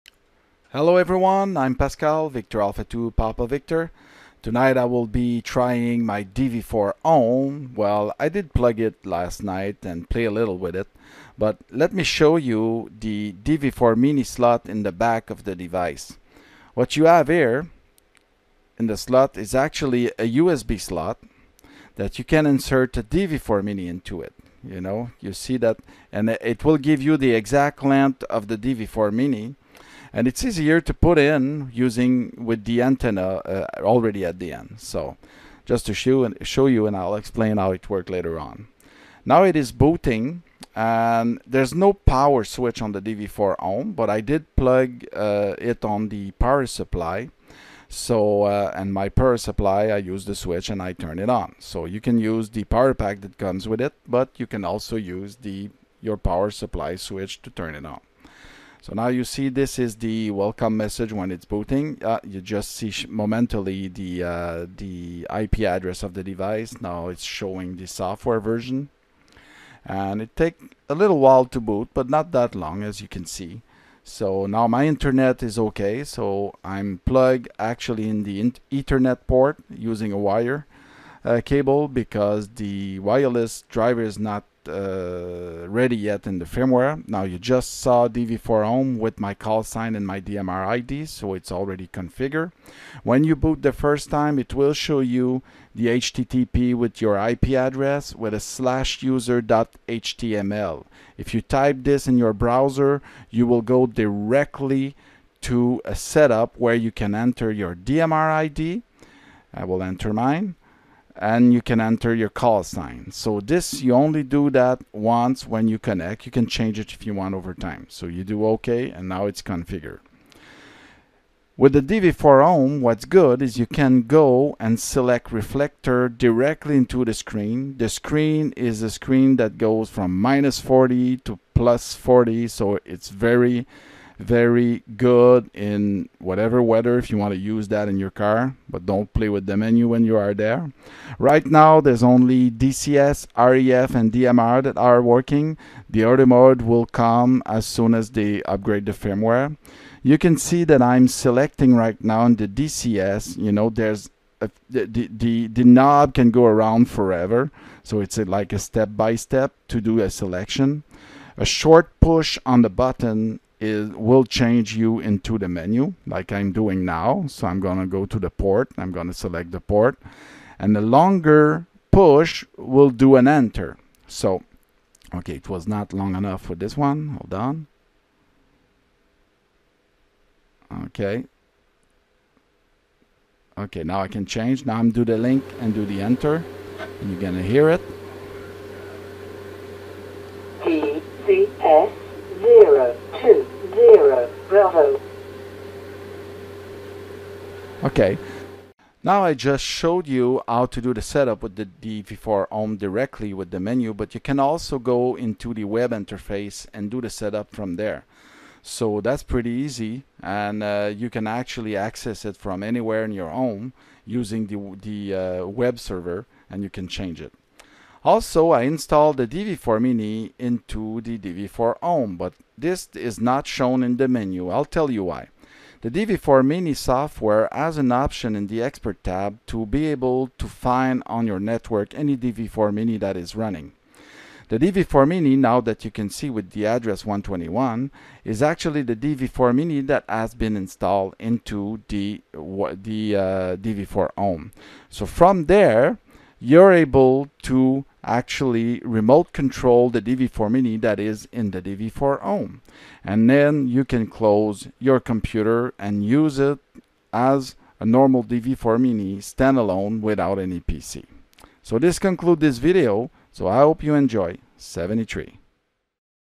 Part 1 - DV4home - Basic setup and menu navigation
This is the part 1 video of the DV4home, showing the basic setup and menu navigation.